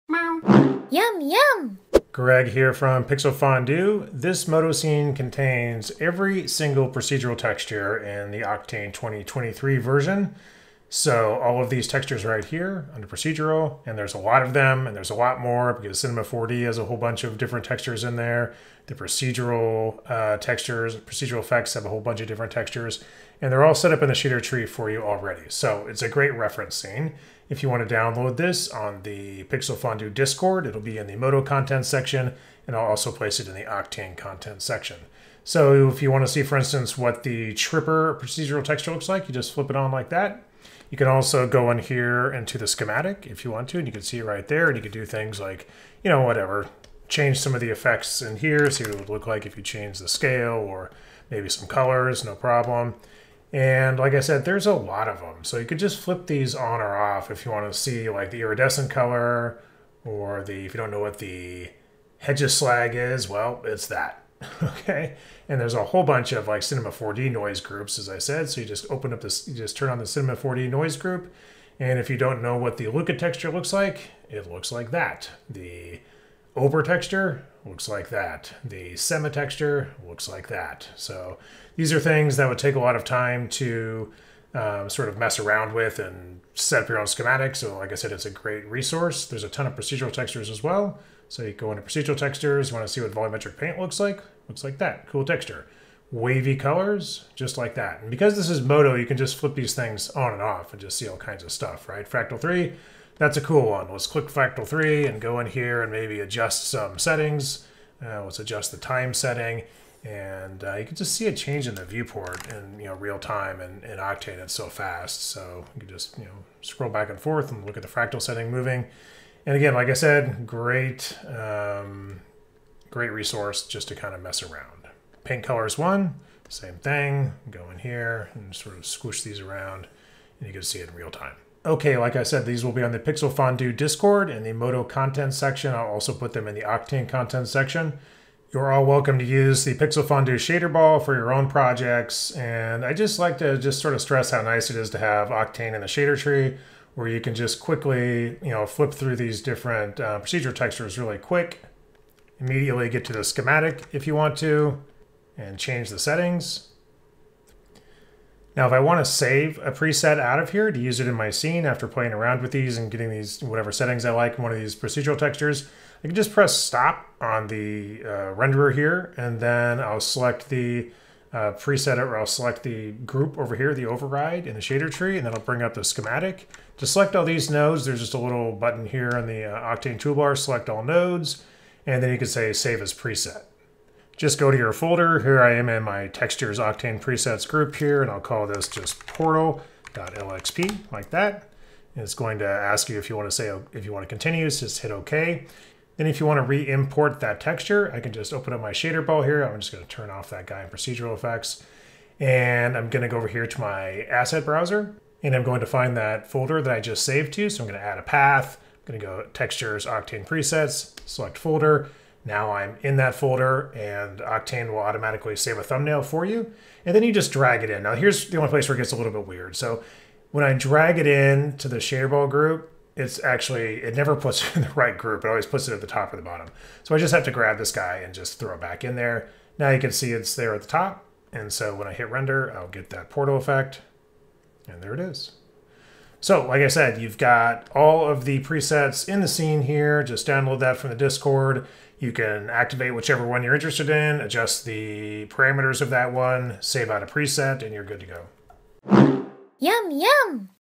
All 2023 Octane Procedural Textures
Go to the Pixel Fondue Discord (link below) to download the Modo scene with all Octane 2023 Procedural textures.

You can use this scene for reference and so adjust parameters and save out presets.  Feel free to use the Pixel Fondue shaderball scene for whatever you want.